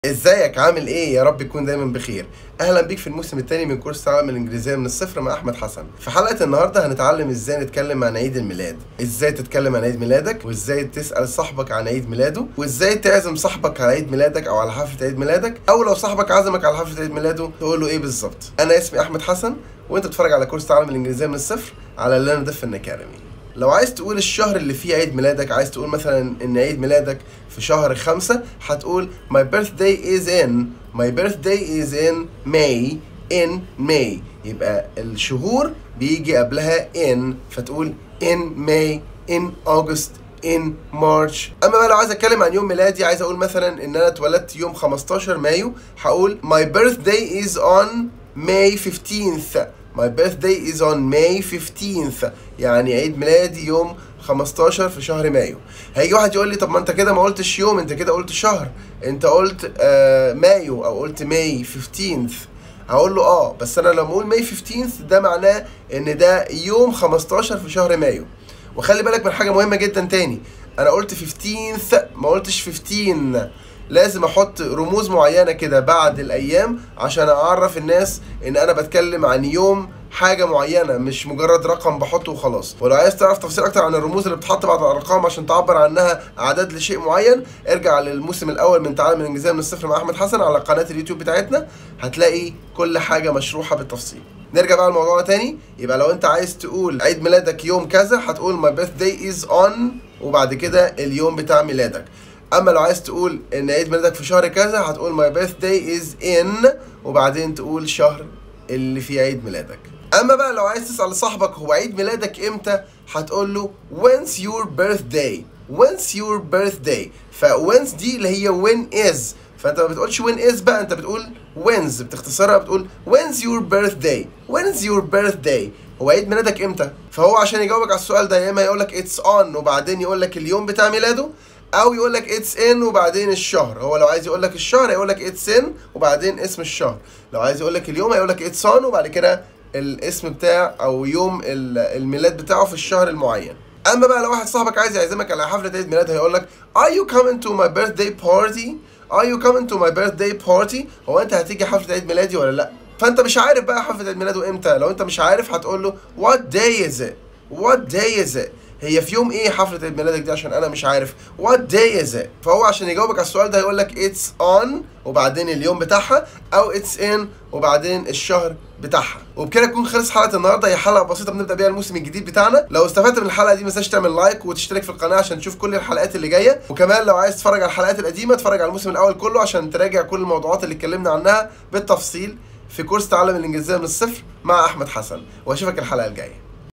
ازاي اتعلم انجليزي ؟ - ازاي تتكلم عن عيد ميلادك بالإنجليزي؟ - كورس شامل للانجليزية
في الحلقة دي هنتعلم ازاي نتكلم عن عيد ميلادنا بالإنجليزي ، وازاي نسأل أصحابنا عن أعياد ميلادهم ، وازاي نعزم أصحابنا على حفلات أعياد ميلادنا